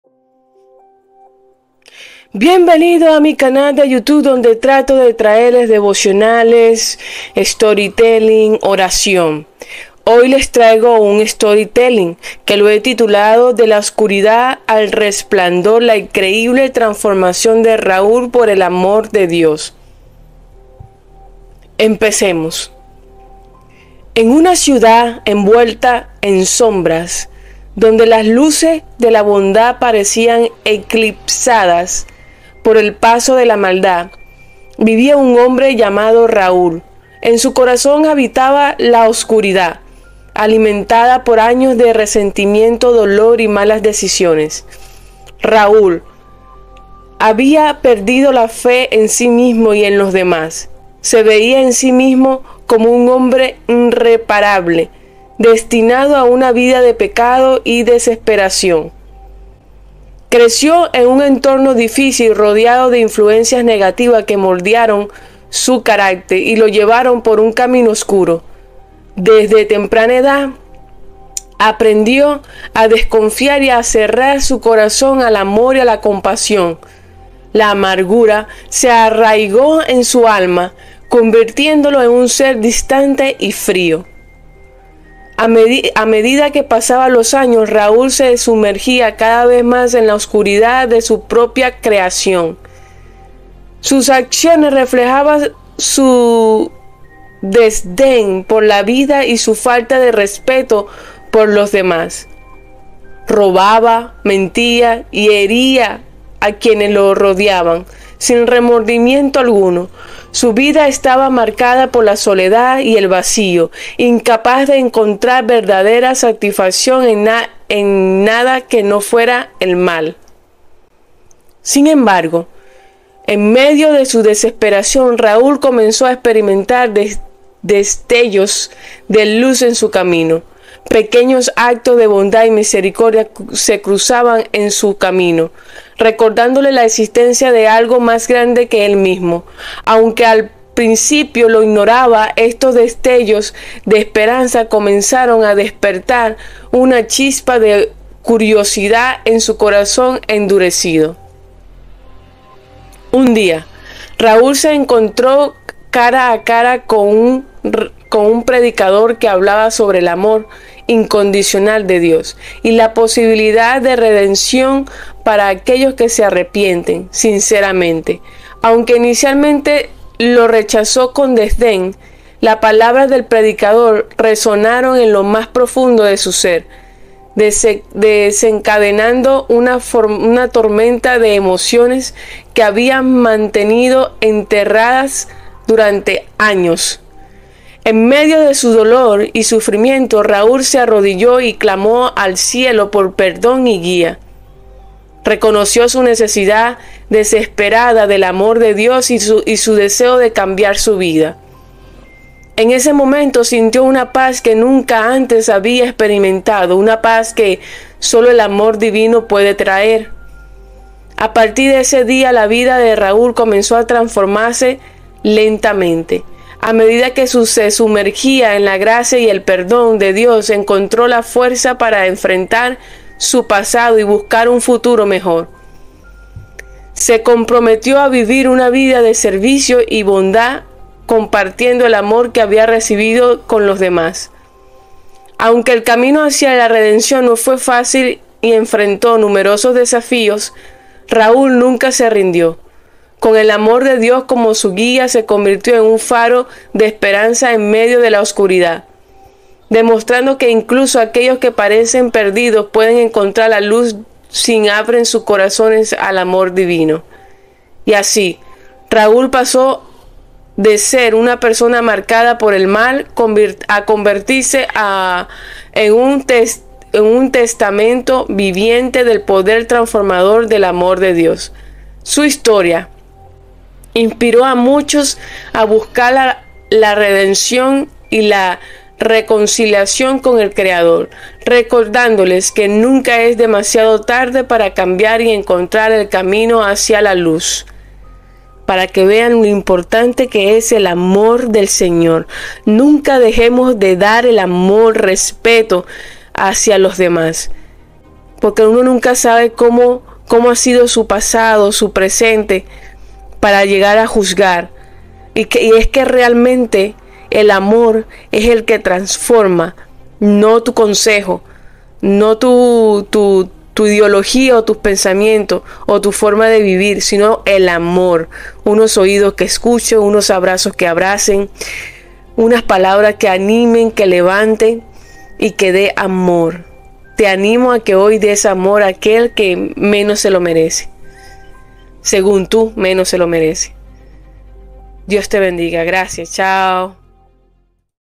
Necesitas el Amor de Dios?
🌟 Sumérgete en la cautivadora historia de Raúl, un hombre que una vez estuvo perdido en la oscuridad de su propio ser, pero encontró la luz a través del amor de Dios. Acompáñalo en su emocionante viaje de redención, donde el perdón divino y la gracia transforman su vida de desesperación y pecado a esperanza y redención. Descubre cómo el poder del amor incondicional de Dios puede cambiar incluso los corazones más endurecidos y guiarlos hacia una nueva vida llena de propósito y significado. Una narrativa conmovedora que te recordará que siempre hay esperanza, incluso en los momentos más oscuros. ¡Prepárate para ser inspirado y conmovido por esta poderosa historia de fe y transformación! 🙏❤️

soporte y apoyo a este canal